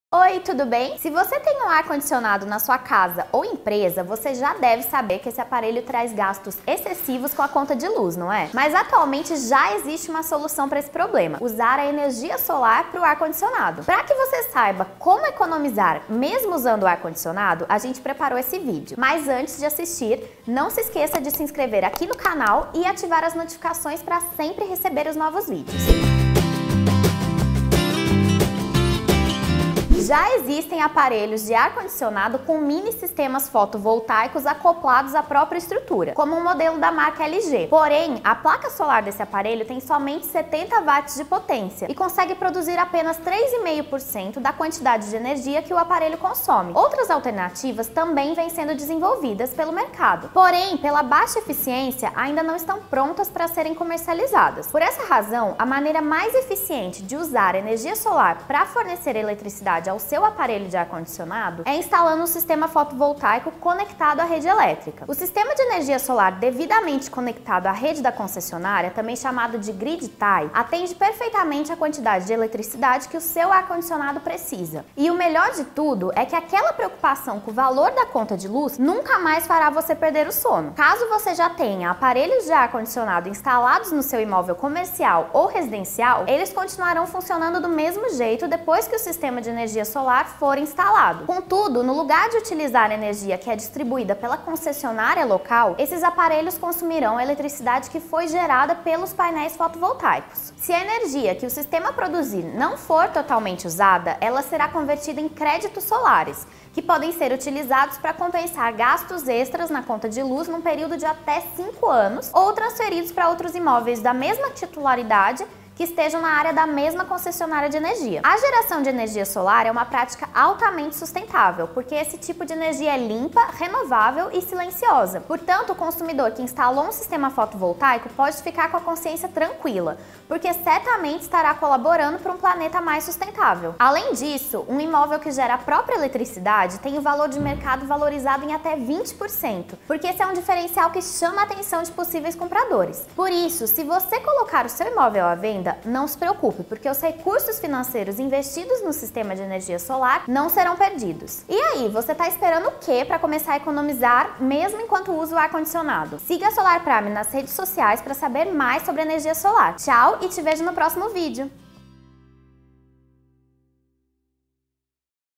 Energia solar para Ar Condicionado
O ar condicionado é o grande vilão da conta de luz? Será? Entenda neste vídeo como usar o seu ar condicionado sem pagar caro no final do mês!

Descubra Quanto Custa Ter Energia Solar em Sua Casa ou Negócio, simples rápido e sem compromisso: